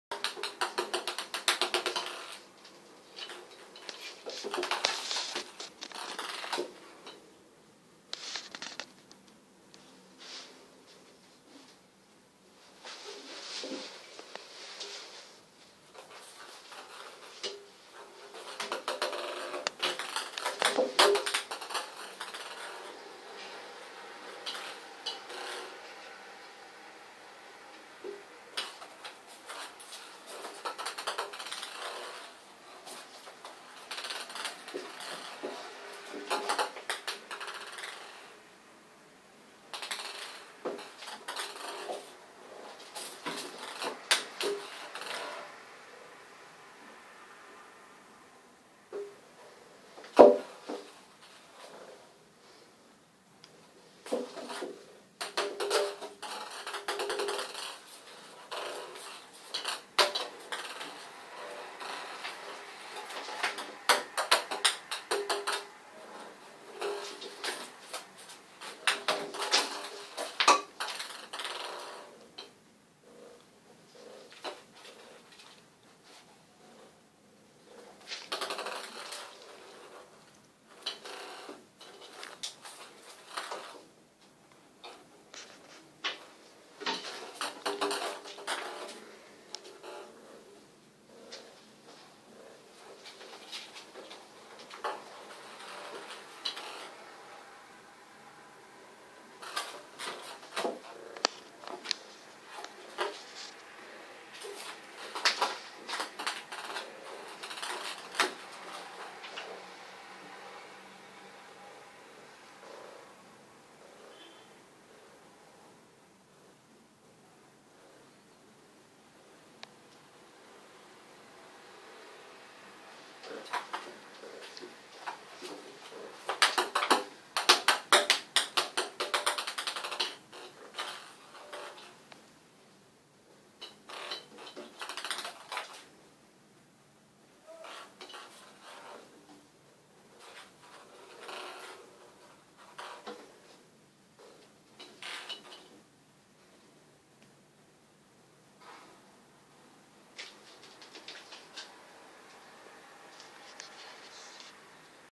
Two cats, a bathtub and a ping-pong ball - 2013.09.27
My 6-month old cats, batting a ping-pong ball around the bathtub. ^_^;;

Δύο γατόνια παίζουν με ένα μπαλάκι του ping-pong μέσα στη μπανιέρα. Η Milou περιμένει ανυπόμονα τη σειρά της και ο Spock γουργουρίζει σαν κομπρεσεράκι από τη χαρά του! =)